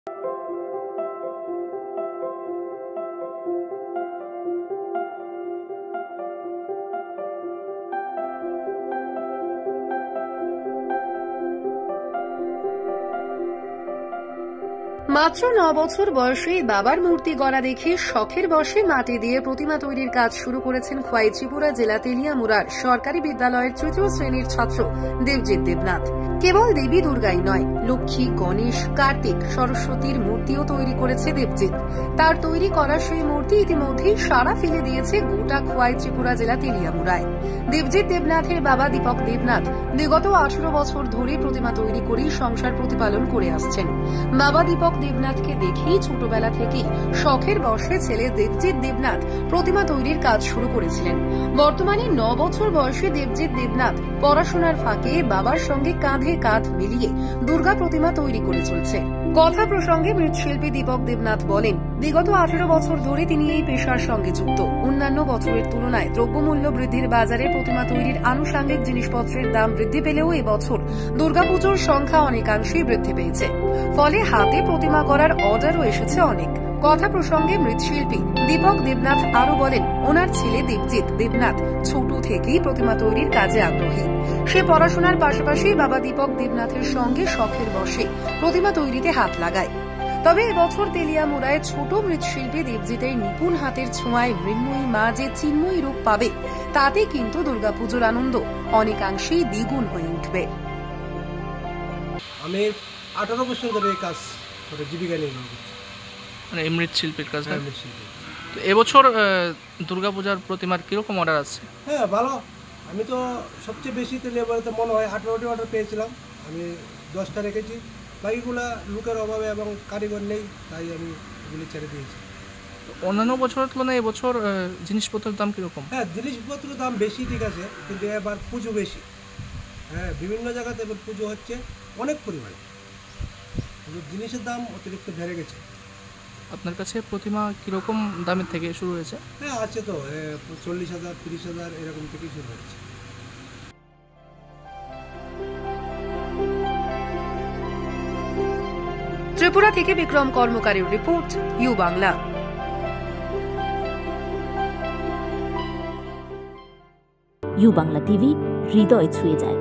Tripura | নিজের হাতে দুর্গা প্রতিমা তৈরি করে তাক লাগিয়ে দিয়েছে তৃতীয় শ্রেণীর ছাত্র | U Bangla TV
মাত্র নয় বছর  বয়সেই  বাবার মূর্তি গড়া দেখে শখের বসে মাটি দিয়ে প্রতিমা তৈরির কাজ শুরু করেছেন খোয়াই ত্রিপুরা জেলা তেলিয়ামুড়ার সরকারি দ্বাদশ শ্রেনী বিদ্যালয়ের তৃতীয় শ্রেণীর ছাত্র দেবজিৎ দেবনাথ। কেবল দেবী দুর্গাই নয় লক্ষ্মী,গণেশ, কার্তিক, সরস্বতী মূর্তি ও তৈরি করেছে দেবজিৎ। তার তৈরি করা সেই মূর্তি ইতিমধ্যেই সাড়া ফেলে দিয়েছে গোটা খোয়াই ত্রিপুরা জেলা তেলিয়ামুড়ায়। দেবজিৎ দেবনাথের বাবা দীপক দেবনাথ বিগত ১৮ বছর ধরে  প্রতিমা তৈরি করেই সংসার প্রতিপালন করে আসছে। বাবা দীপক দেবনাথকে দেখেই ছোটবেলা থেকেই শখের বসে ছেলে দেবজিৎ দেবনাথ প্রতিমা তৈরি কাজ শুরু করেছিলেন। বর্তমানে নয় বছর বয়সী দেবজিৎ দেবনাথ পড়াশুনার ফাঁকে বাবার সঙ্গে কাঁধে কাঁধ মিলিয়ে দূর্গা প্রতিমা তৈরি করে চলছে।কথা প্রসঙ্গে মৃৎশিল্পী দীপক দেবনাথ বলেন, বিগত ১৮ বছর ধরে তিনি এই পেশার সঙ্গে যুক্ত। অন্যান্য বছরের তুলনায় দ্রব্যমূল্য বৃদ্ধির বাজারে প্রতিমা তৈরীর আনুষাঙ্গিক জিনিসপত্রের দাম বৃদ্ধি পেলেও এবছর দুর্গাপুজোর সংখ্যা অনেকাংশই বৃদ্ধি পেয়েছে। ফলে হাতে প্রতিমা গড়ার অর্ডারও এসেছে অনেক। কথাপ্রসঙ্গে মৃৎশিল্পী দীপক দেবনাথ আরও বলেন, উনার ছেলে দেবজিৎ দেবনাথ ছোট থেকেই প্রতিমা তৈরীর কাজে আগ্রহী। সে পড়াশোনার পাশাপাশি বাবা দীপক দেবনাথের সাথে শখের বসে প্রতিমা তৈরিতে হাত লাগায়। তবে এবছর তেলিয়ামুড়ায় ছোট্ট মৃৎশিল্পী দেবজিৎ-এর নিপুন হাতের ছোঁয়ায় মৃন্ময়ী মা যে চিন্ময়ী রূপ পাবে তাতে কিন্তু দুর্গাপুজোর আনন্দ অনেকাংশই দ্বিগুণ হয়ে উঠবে।